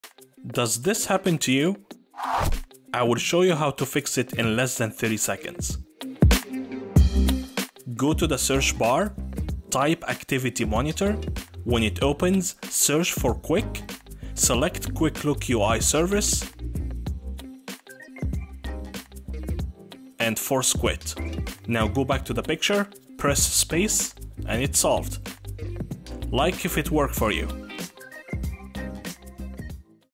How To Fix Quick Look Preview Finder issue on macOS Monterey
On macOS, especially after upgrading to Monterey, Preview sometimes stops showing full window previews for images, PDFs and videos. Instead, it shows this tiny preview window. This is the only fix that worked for me. Let me know if this helped you.